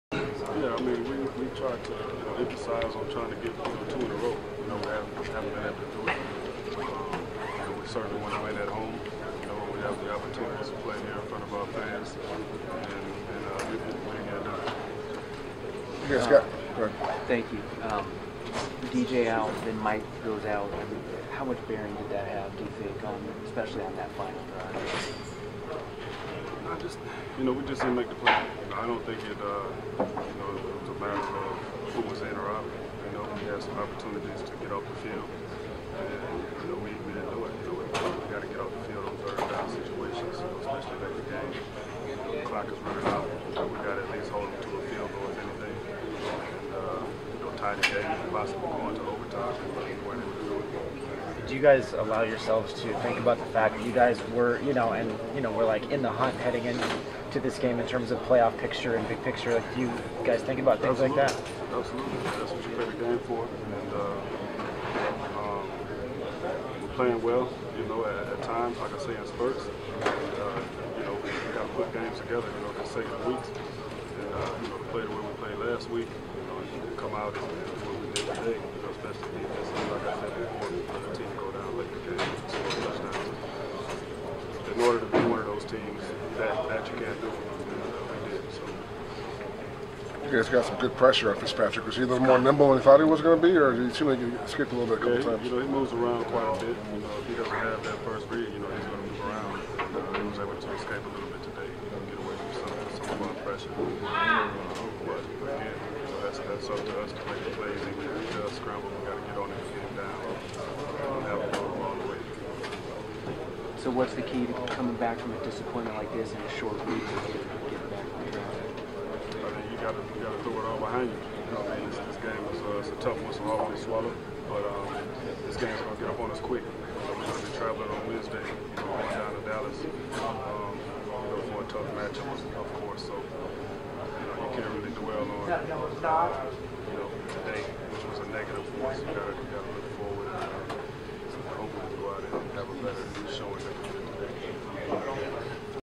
Charles Woodson Postgame 11-24-13
Oakland Raiders safety Charles Woodson speaks to the media following a 23-19 loss to the Tennessee Titans